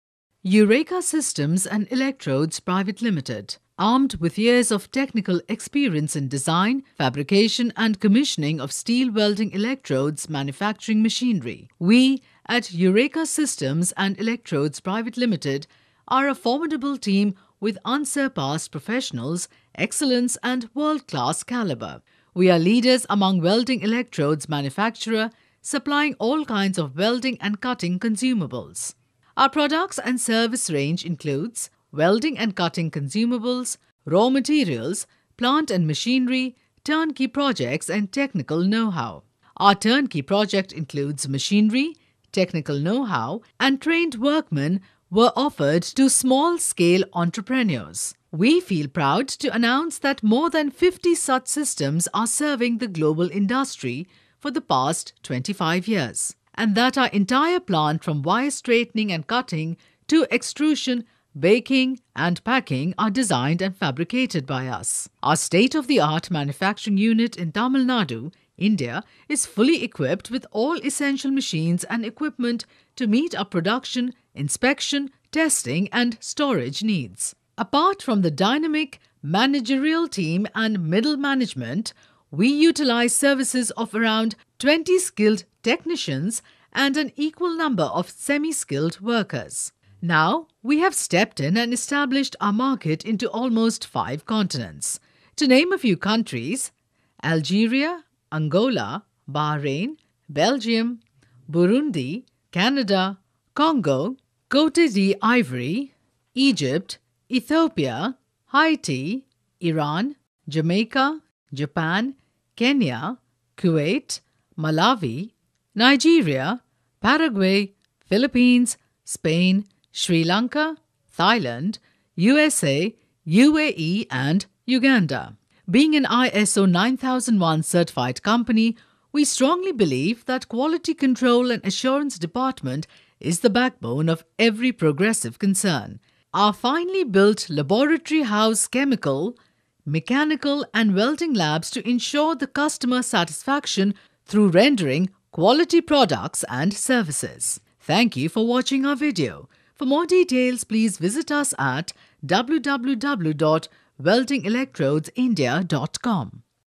Eureka Systems And Electrodes Private Ltd., Coimbatore
Armed with years of technical experience in design, fabrication and commissioning of steel welding electrodes manufacturing machinery. We at Eureka Systems And Electrodes Private Ltd are a formidable team with unsurpassed professionals excellence and world-class caliber. We are leaders among welding electrodes manufacturer, supplying all kinds of welding and cutting consumables.

Our products and service range includes -Welding and Cutting Consumables, Raw Materials, Plant And Machinery, Turnkey Projects & Technical know-how

Our Turnkey project includes machinery, technical know-how, and trained workmen were offered to small-scale entrepreneurs. We feel proud to announce that more than 50 such systems are serving the Global Industry for the past 25 years, and that our entire plant from wire straightening and cutting to extrusion, baking and packing are designed and fabricated by us.

Our state-of-the-art manufacturing Unit at Tamil Nadu, India is fully equipped with all essential machines and equipment to meet our production, inspection, testing and storage needs. Apart from the dynamic managerial team and middle management, we utilize services of around 20 skilled technicians and an equal number of semi skilled workers.

Now we have stepped in and established our market into almost five continents.

Being an ISO 9001 certified company, we strongly believe that quality control and assurance department is the backbone of every progressive concern. Our finely built laboratory house chemical, mechanical and welding labs to ensure the customer satisfaction through rendering quality products and services.